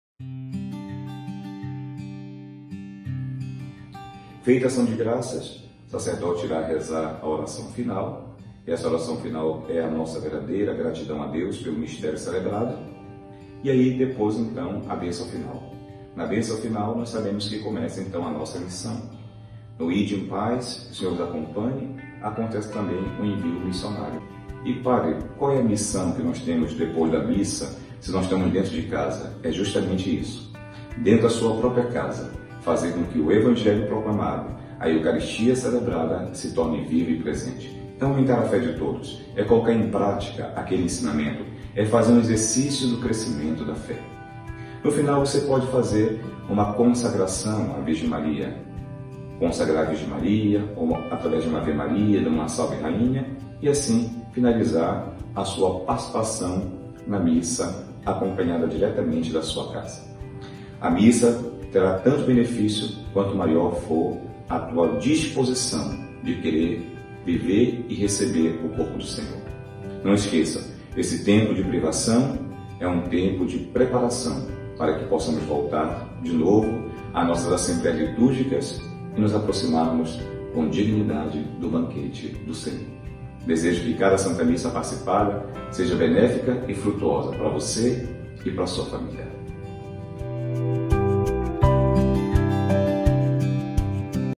Missão na igreja doméstica - Padre Agostinho
Padre Agostinho explica e orienta qual a missão dos cristão durante o período do isolamento social na sua casa (igreja doméstica). Dê o play e assista.
Participe das celebrações eucarísticas aqui em nosso canal, em nossa página no Facebook